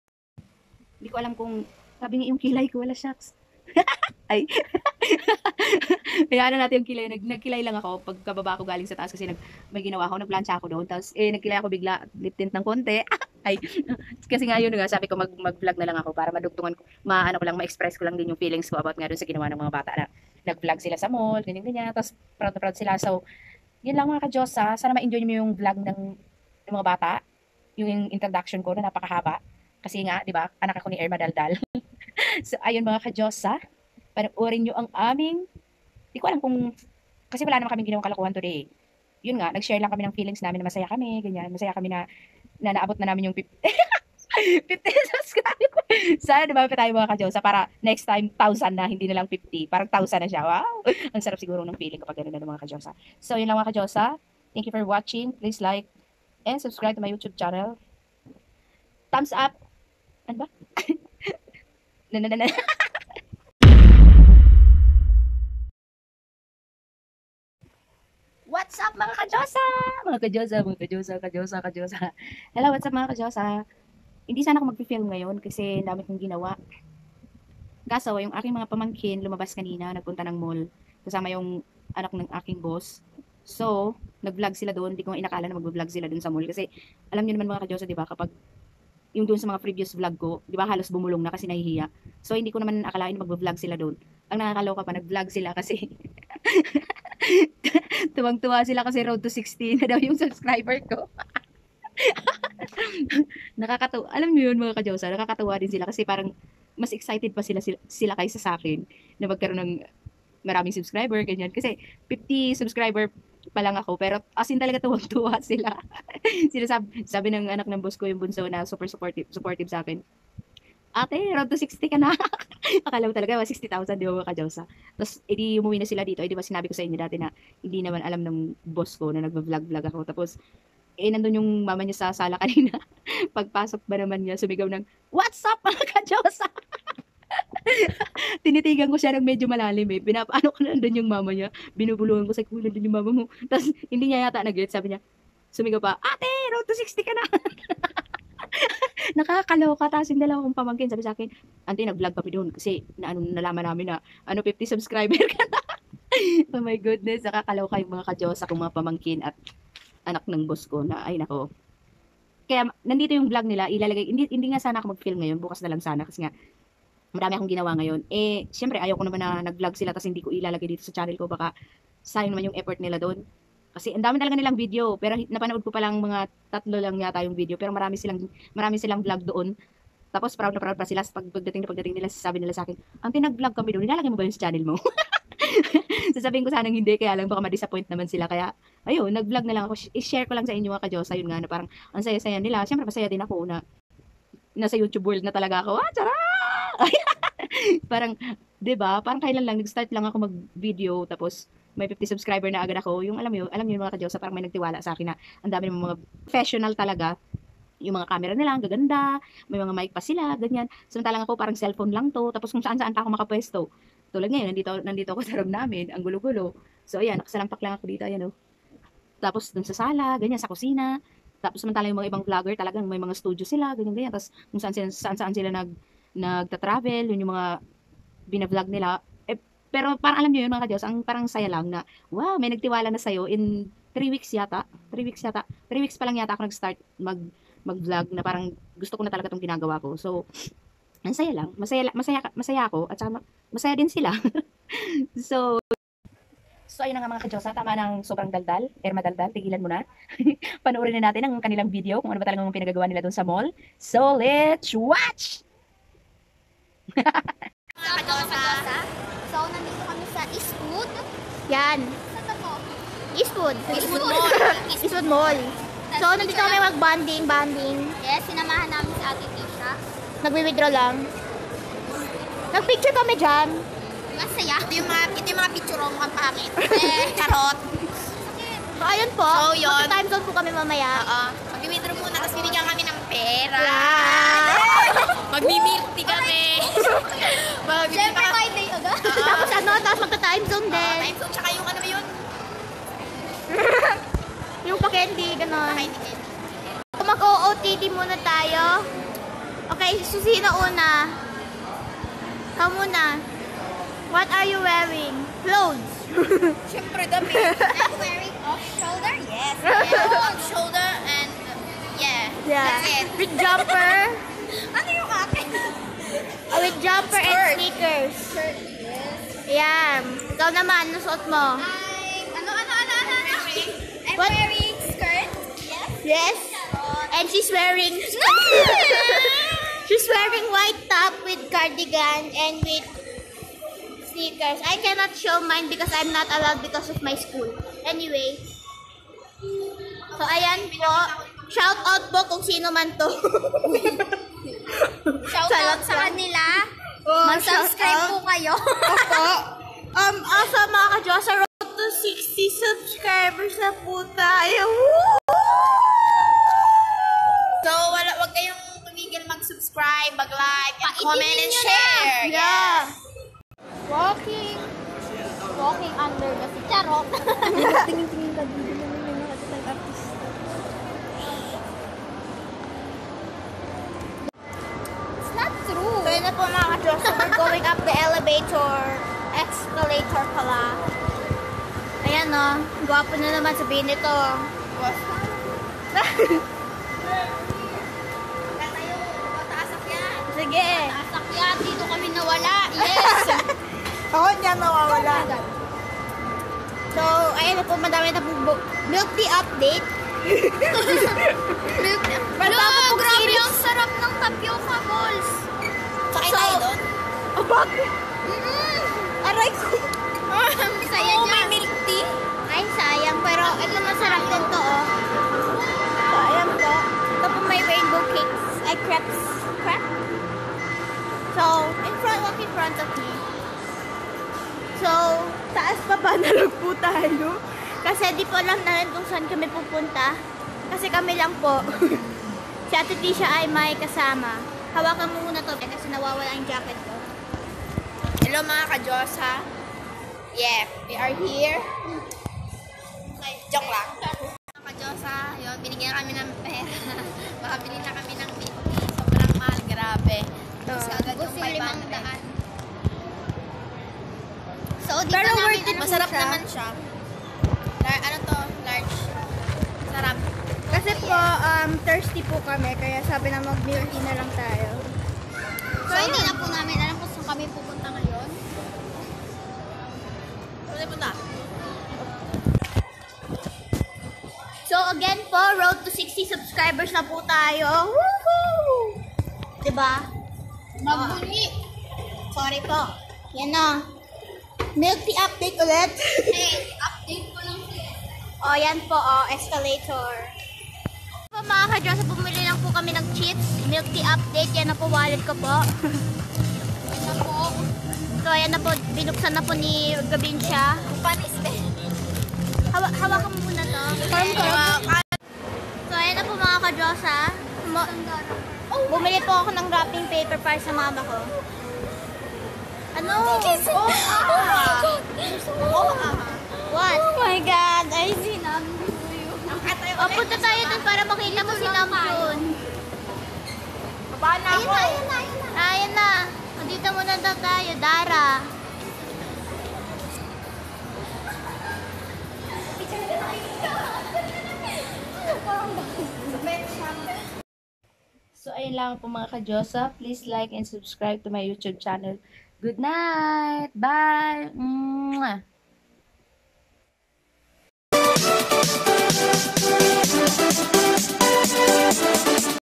Masaya lang po mga kadyosa, 50 subscribers 😂😂 happyus happyme letsallbehappy AireenG/Vlog
Dahil sa 50 sub napa vlog tuloy ako,  at dahil din sa effort ng 3 batang itey.  Thank you mga kadyosa.